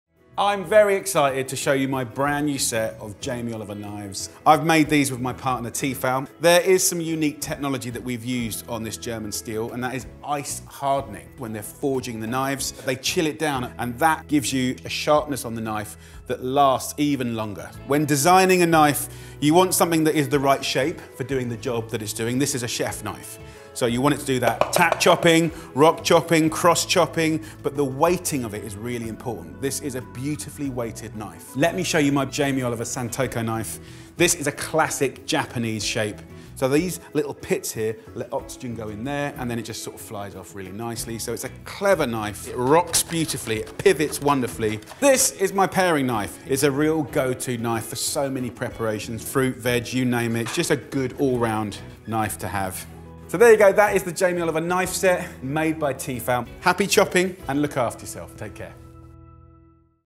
How to sharpen your knife skills in 60 seconds | Jamie Oliver
Check out the new kitchen knife set from Jamie Oliver and Tefal! Brush up on your skills and learn how to get the most out of each knife.

Keen to get your hands on them? Could come in handy with fathersday around the corner…
So geht's: Verbessere deine Schneidekünste in nur 60 Sekunden!

x